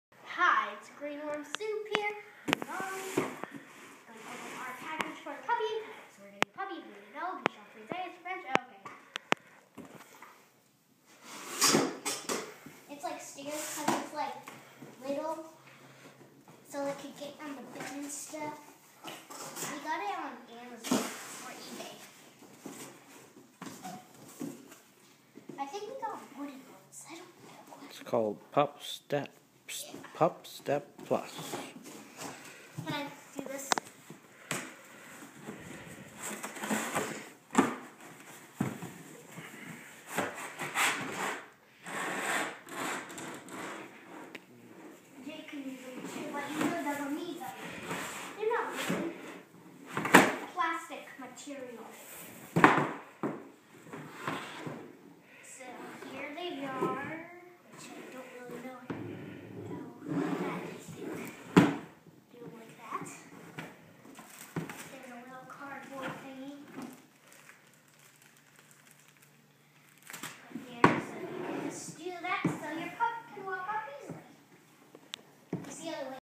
Unboxing Solvit PupStep Pet Stairs
Pet Stairs just arrived for our future puppy, Everest.  Purchased from Amazon.

Join the millions who are helping me raise money for a new gopro camera.  Thanks!